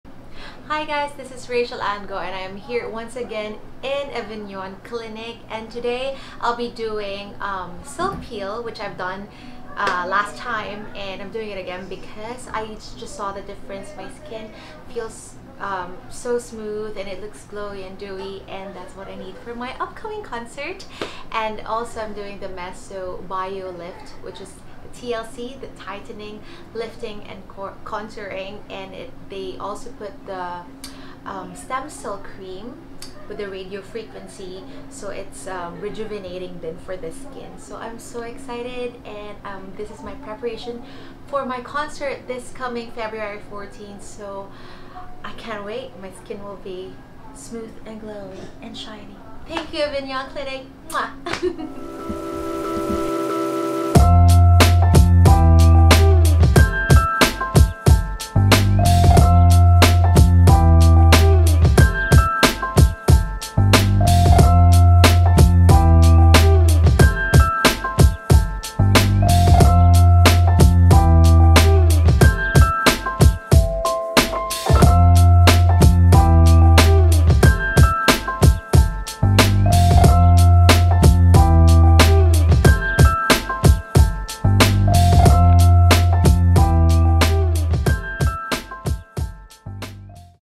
Avignon Mesobiolift Skin Reset
Mesobiolift Skin Reset is the first lifting facial that uses Endothermal system with radiofrequency and micro galvanic current. We apply plant-based stem cell emulsion (that's the white cream that you see) which nourishes the skin right to its core. This a powerful anti-ageing treatment with lifting effect. It redefines the facial contours targeting specific areas like, laugh lines, deep wrinkles, double chins, sagging face, bags under eyes.
The most common patient feedback that we get right after the treatment is--"its like I have slept for full 8 hours."
Downtime: No Downtime
Duration: 30 to 40 minutes
Visible results can be observed after only one session, we offer this therapy as a course of treatments and recommend three to six sessions for best results.

The Mesobiolift device and Stem Cell Emulsion are imported from Spain. You can be sure that this has passed strict European Standards and locally approved by the FDA.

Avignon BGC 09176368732
Avignon QC (across ABS CBN) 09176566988